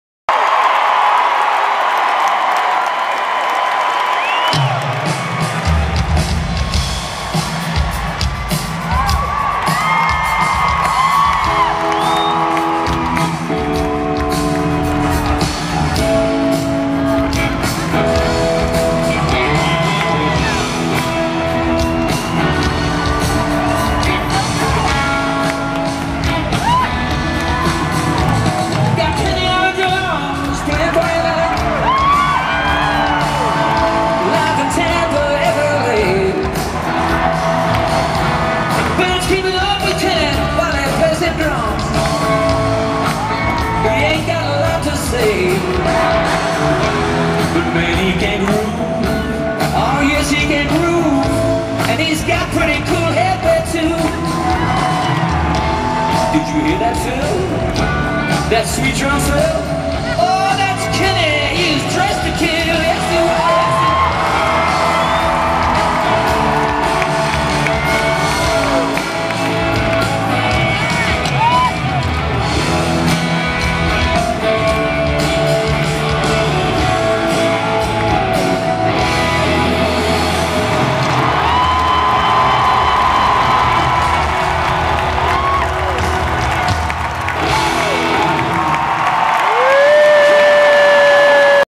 A fan plays drum with Keith Urban in Tampa
When your wife holds up a sign asking for you to play drums with Keith Urban...Mid-Florida Amphitheatre Tampa 6-17-22